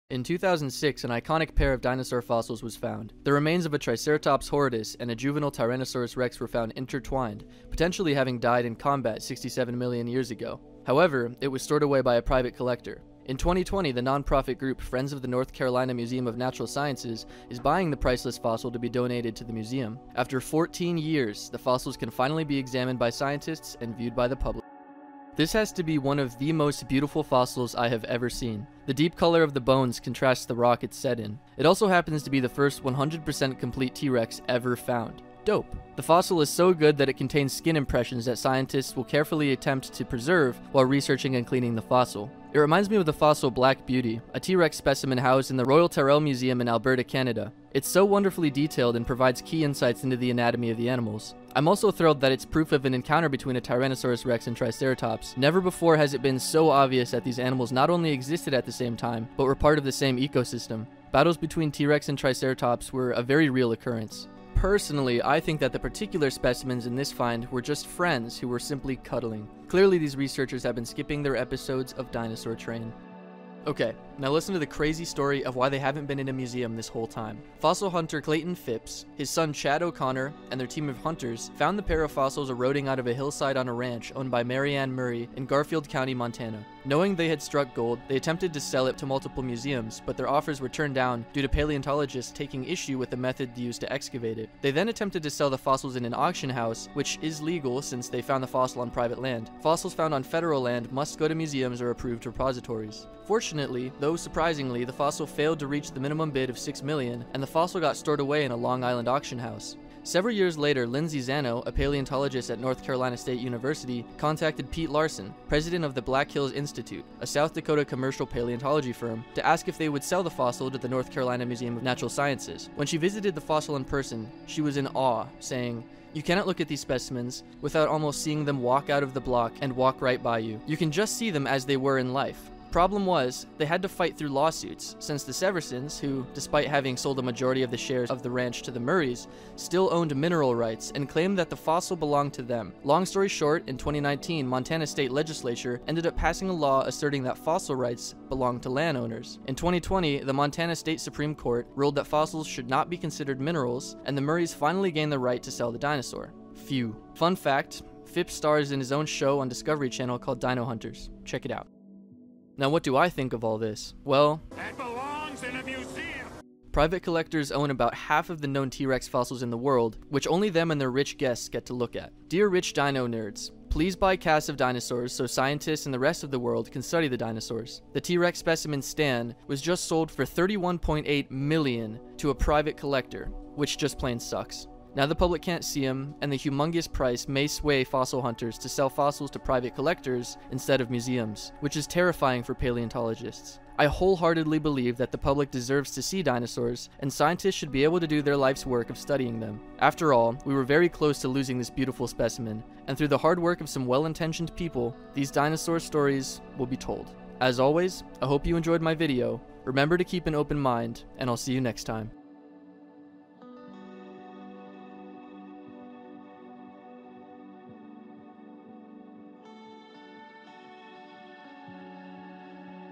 New Fossil: Tyrannosaurus vs Triceratops Fatal Fight
A new fossil of a fatal fight between a Tyrannosaurus rex and Triceratops horridus has recently been recovered to be researched and put on display at the North Carolina Museum of Natural Sciences. Surprise: it's beautiful.

It's the place for dinosaur news and discussions, video recommendations, and community events.

Art Credits:
Triceratops - Science Photo Library, Albany Stock Photos
4-year-old T. rex. Illustration - Zhao Chuang
Juvenile Tyrannosaurs attacking Triceratops - Anthony Hutchings
T. rex vs Triceratops - Phil Wilson
T. rex on top of Triceratops - RJ Palmer